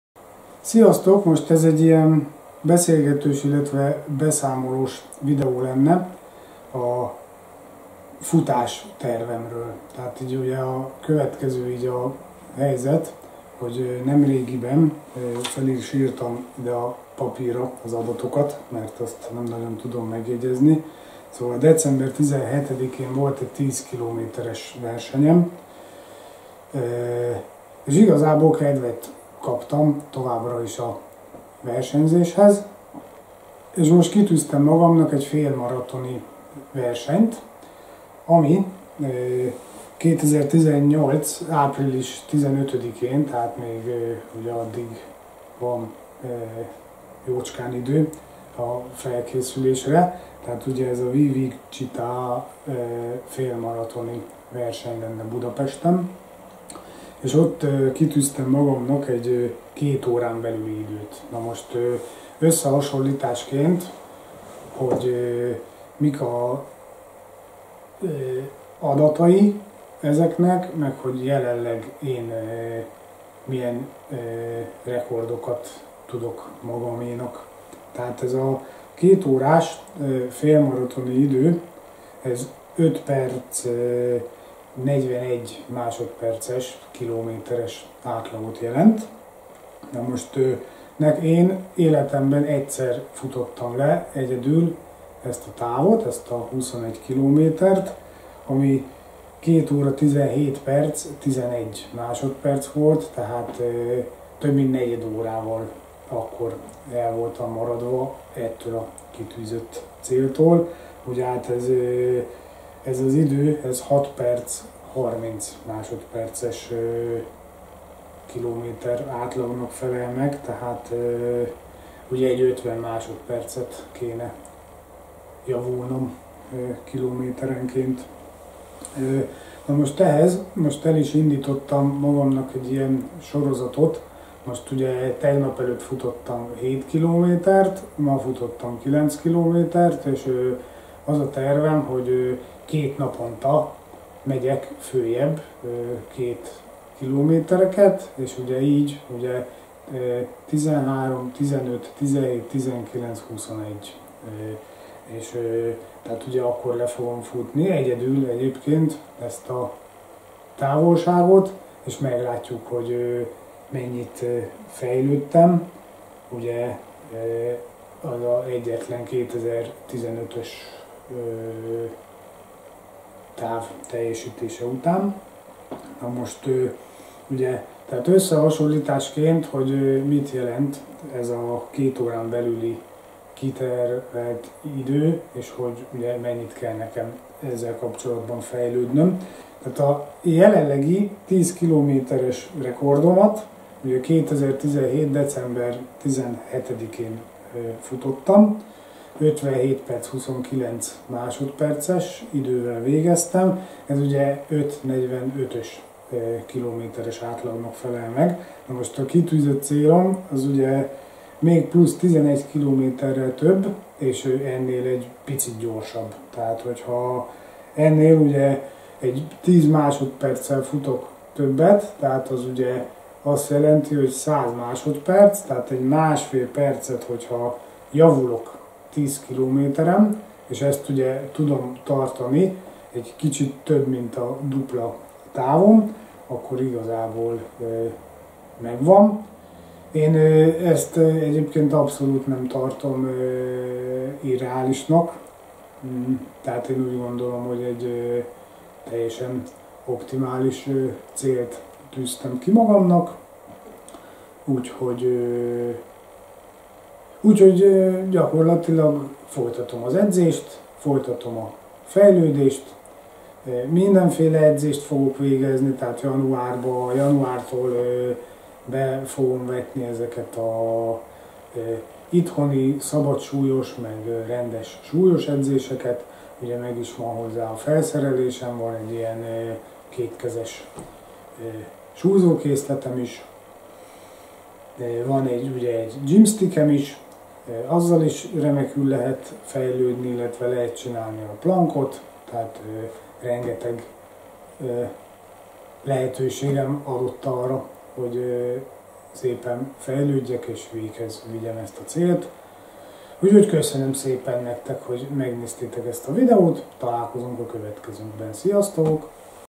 Futás félmaraton tervem
Bemutatom a tervemet : félmaraton felkészülésem